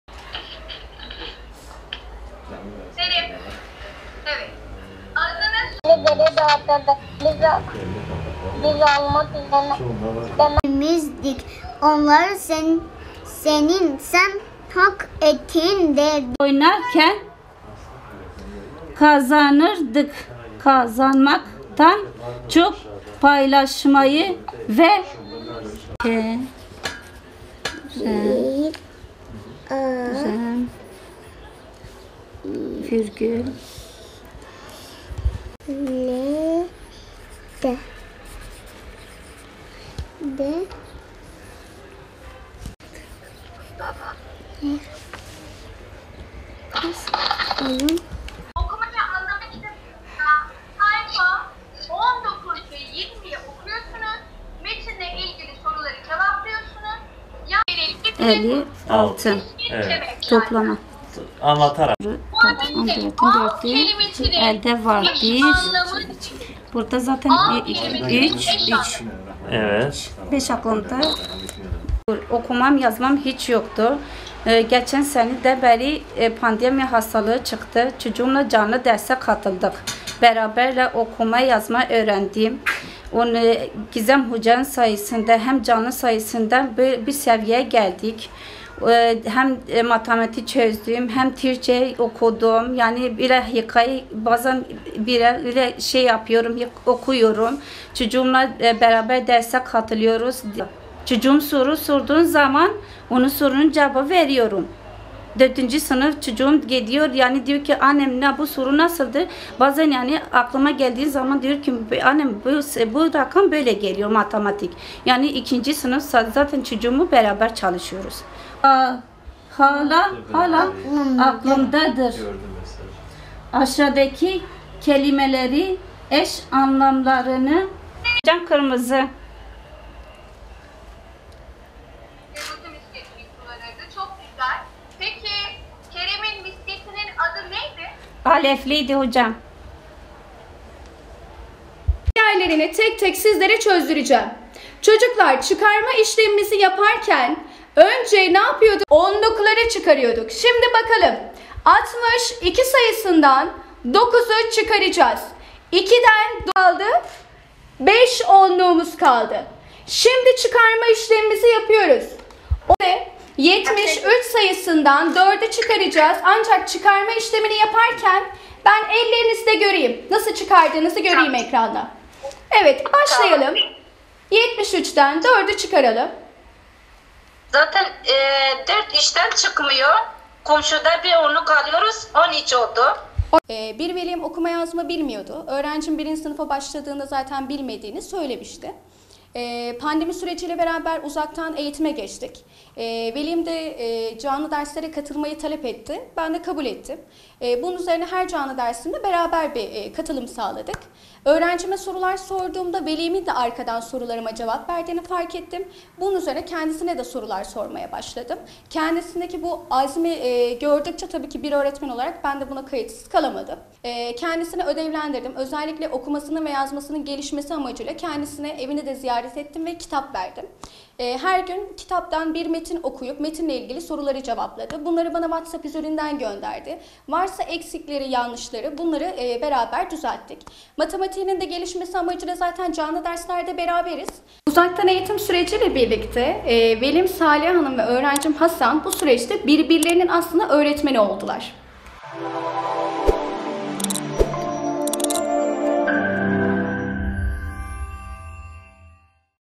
Şırnaklı anne oğluyla beraber EBA üzerinden okuma yazma öğrendi
​Şırnak’ın Silopi ilçesinde 5 çocuk annesi Saliha Uygur, oğlunun EBA üzerinden canlı derslerine katılıp beraber okuma yazma öğrenerek Covid-19 salgını sürecinde evde kalmayı fırsata çevirdi.

Değerli İzleyicilerimiz;

TÜRKÇE

KÜRTÇE-KURMANCI

ARAPÇA

İNGİLİZCE

FARSÇA

KÜRTÇE-SORANİ